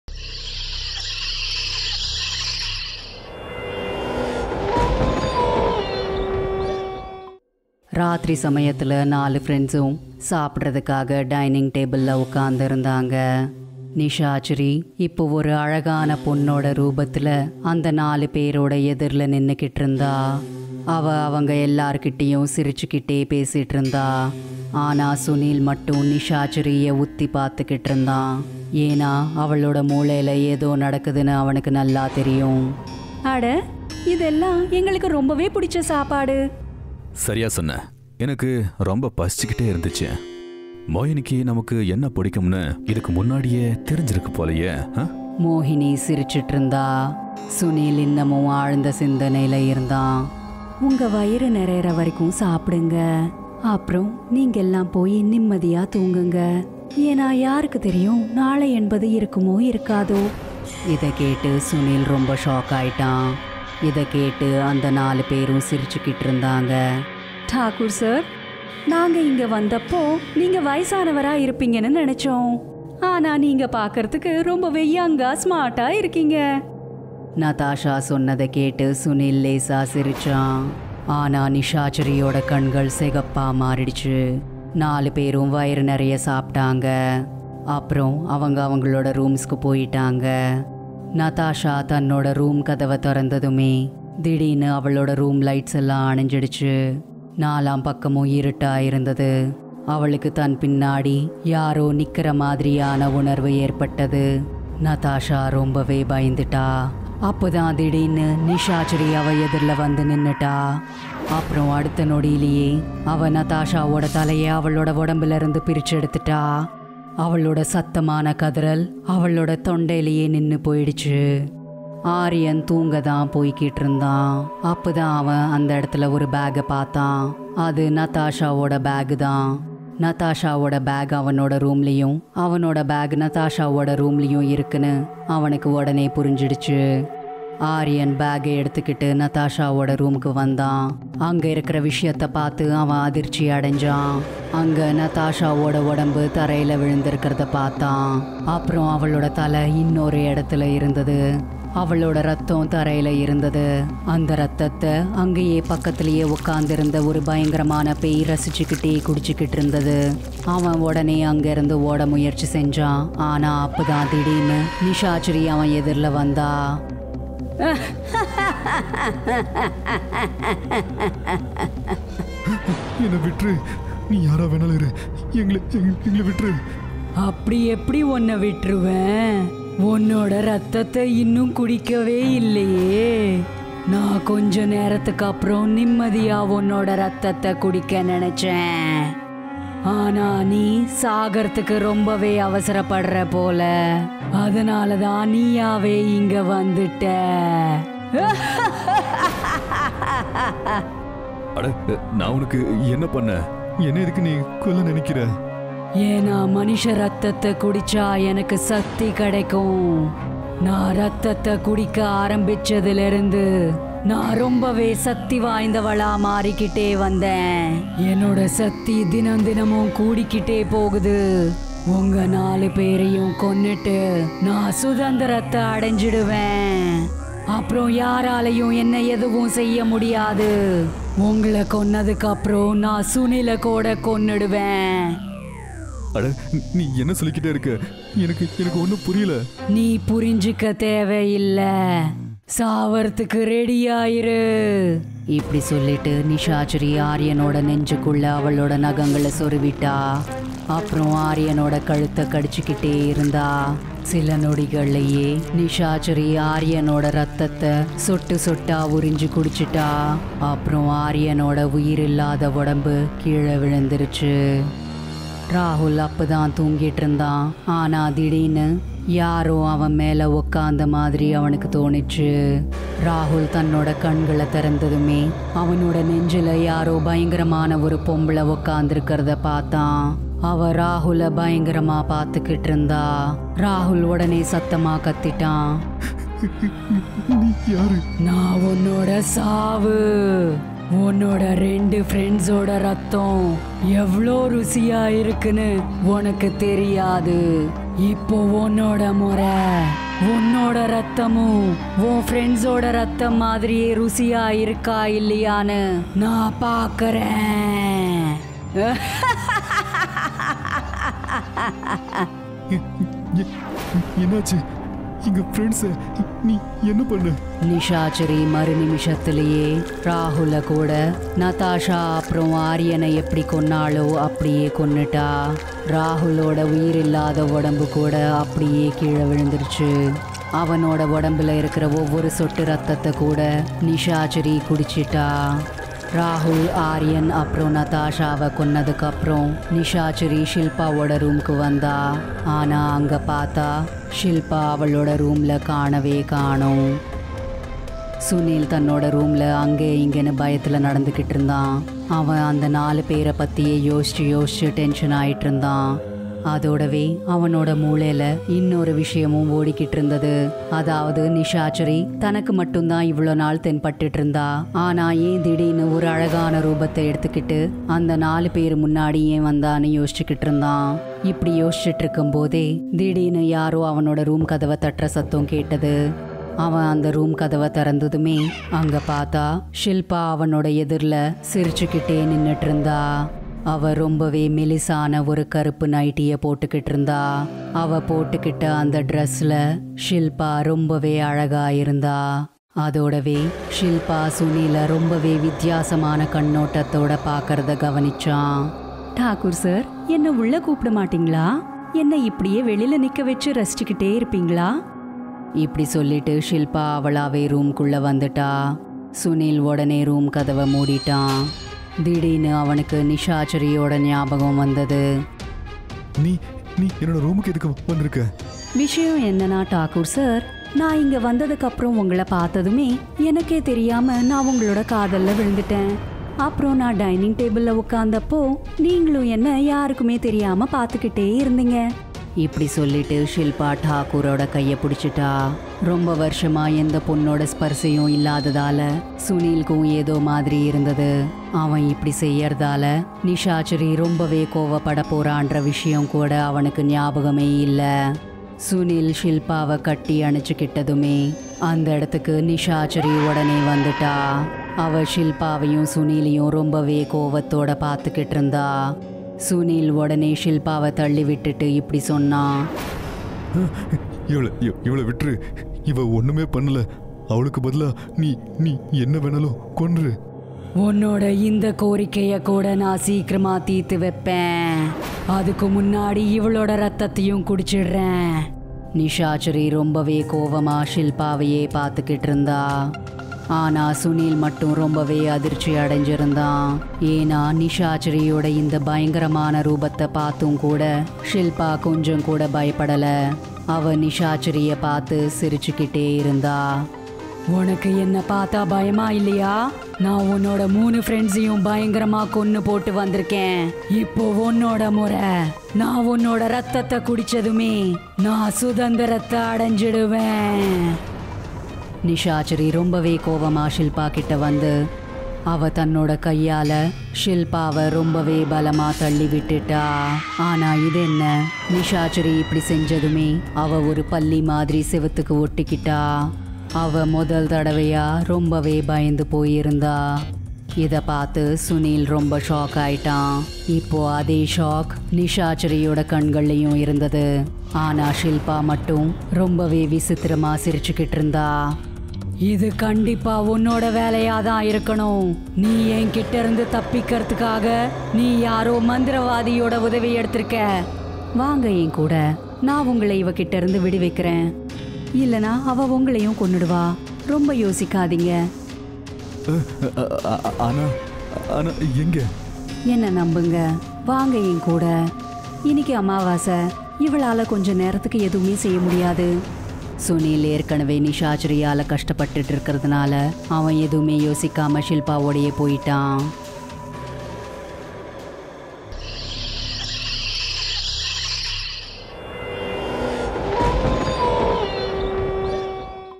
நிஷாச்சாரி சூனியக்காரி Part 4 | Stories in Tamil | Tamil Horror Stories
நிஷாச்சாரி சூனியக்காரி Part 4 | Stories in Tamil | Tamil Horror Stories | Tamil Stories | Bedtime Stories

Story :- The Nishachari Witch Part 4